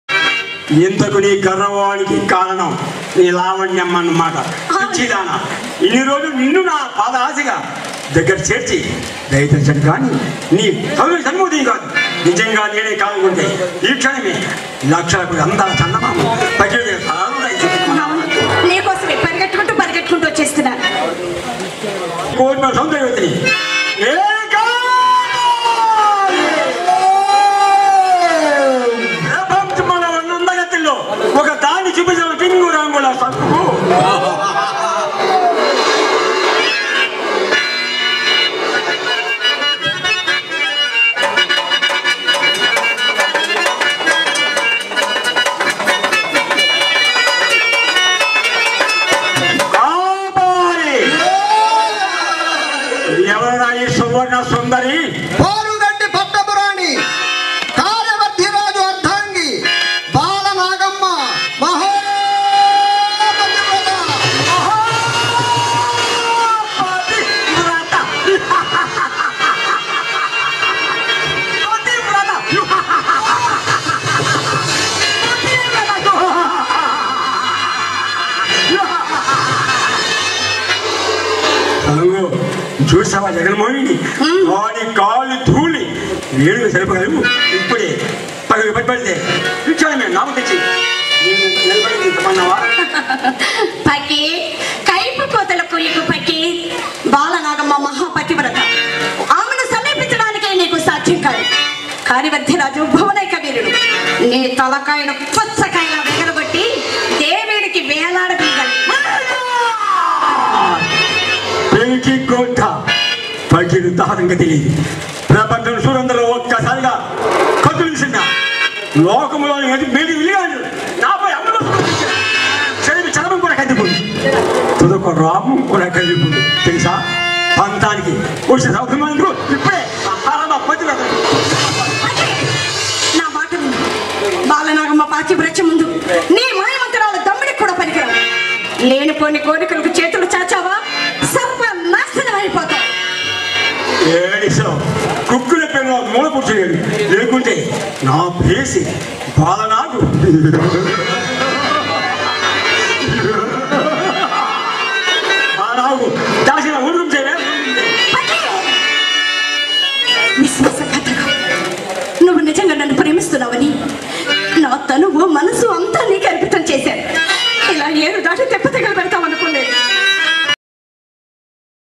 బాలనాగమ్మ/మాయల ఫకీరు/ సంగు డైలాగ్స్ BALANAGAMMA NATAKAM || PHAKIR SANGU DIALOGUES
BALANAGAMMA NATAKAM
PHAKIR SANGU  DIALOGUES
Balanagamma :  surabhi suvarna
Phakir                   bathina nageswar rao

మాయల పకీర్ :  శ్రీ బత్తిన నాగేశ్వరరావు
సంగు               : శ్రీమతి సురభి సువర్ణ

సంగీత సారధ్యం - ఆర్కెస్ట్రా :
శ్రీ కలిగట్ల వెంకటరమణ, రాజమహేంద్రవరం